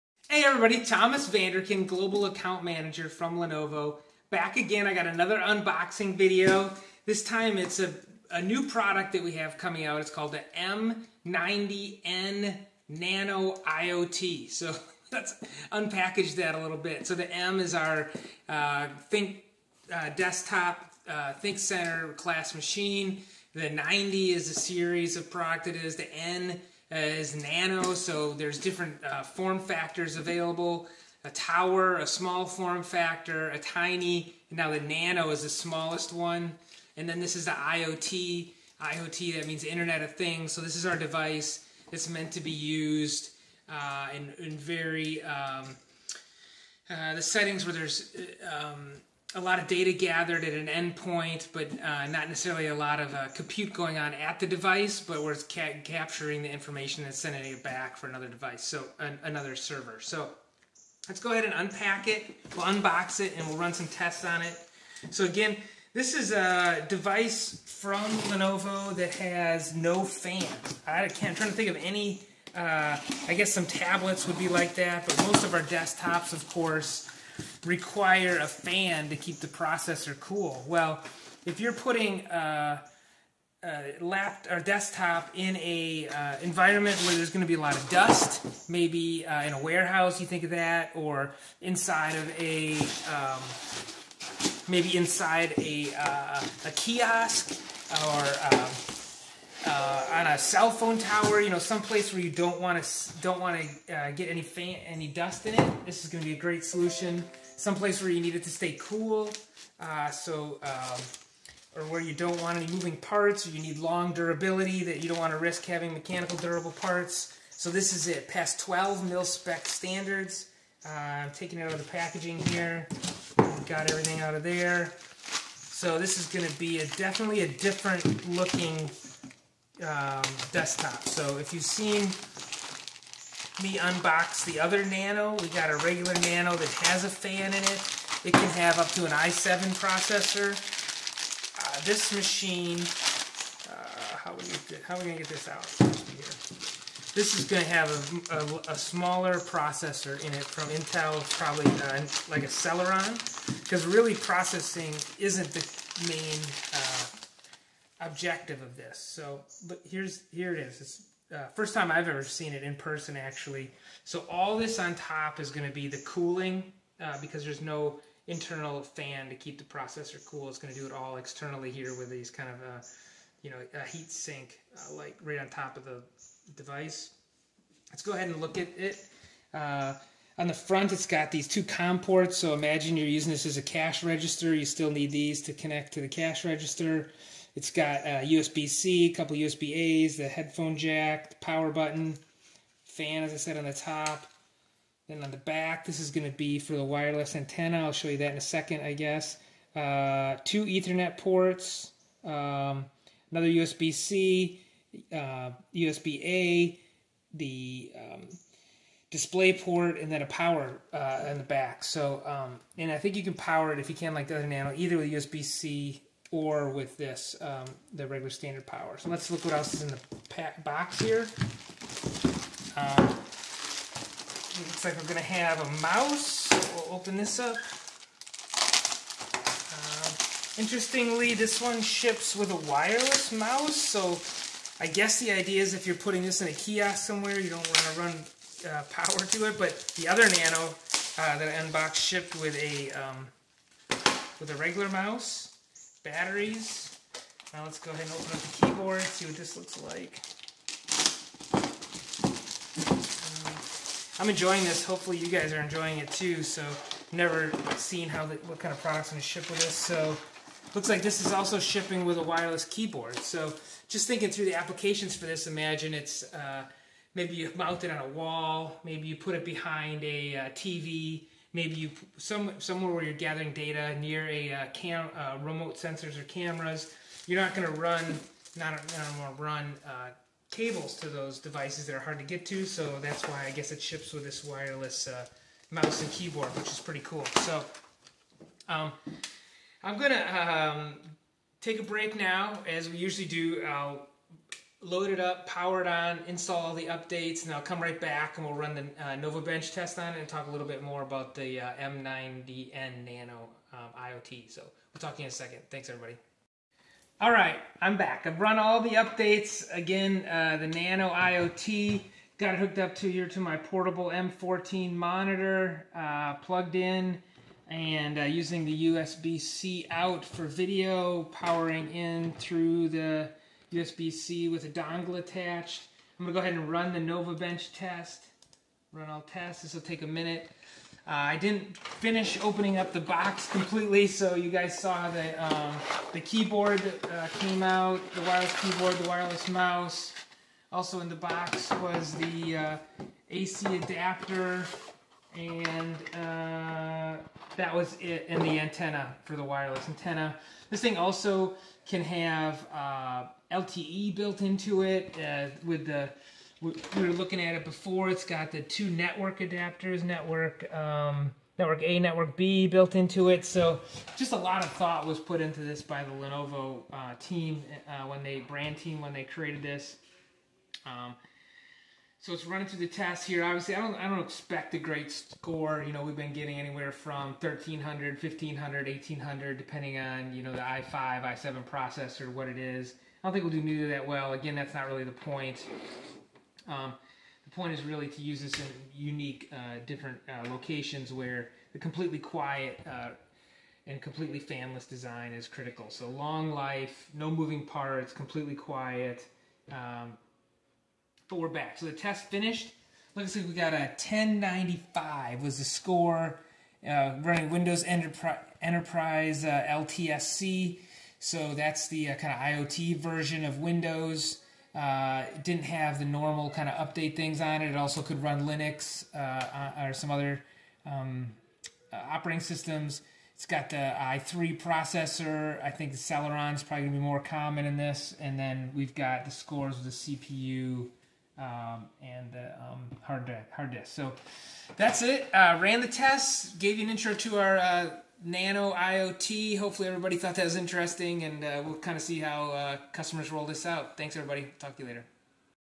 Unboxing the Lenovo ThinkCentre M90n Nano IoT
So, today I will unbox the Lenovo ThinkCentre M90n Nano IoT.  Everyone in the IT business is talking about IoT (Internet of Things).  Basically, what is means is that every device will be connected to the internet.  Think lightbulbs, cars, pets, etc.
Well, as this advances, all sorts of industries will need new types of devices in new locations (warehouses, factories, kiosks, etc).  Since it has two etherports, some also described it to me as IoT bridge.   We expect customers to be very creative as to where they can use these fanless, completely quite, and very rugged devices.  If you have any thoughts or leadership in IoT, please let me know what ideas you have in the comments.